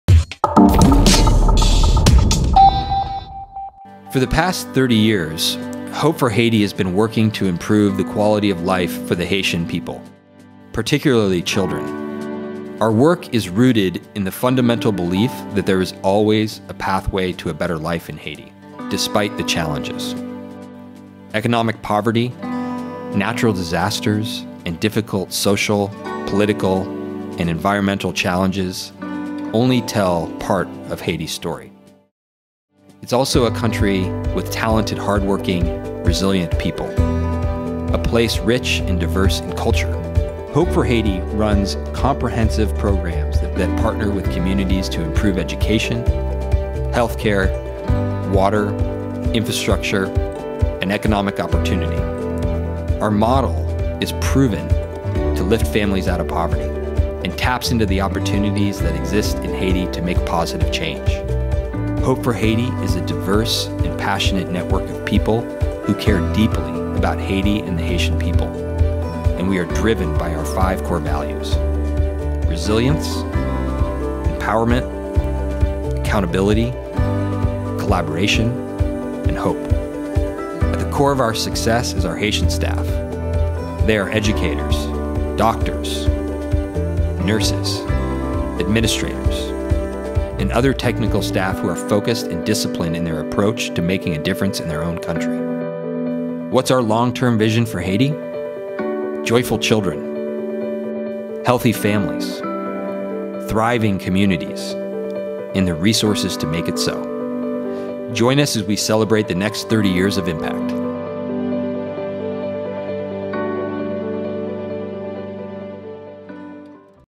WorkProud - Hope For Haiti
As 2021 comes to a close, we would like to take a moment to extend our deep appreciation and gratitude for our clients and partners. We wish you health and happiness in the New Year!

In lieu of traditional client gifts this year, we have made a donation to Hope for Haiti on behalf of our clients to sponsor a mobile health clinic in the village of Morency in 2022.

With this support, Hope for Haiti will be able to provide consultations, basic healthcare services and vaccinations to more than 200 children and their families.

Join us!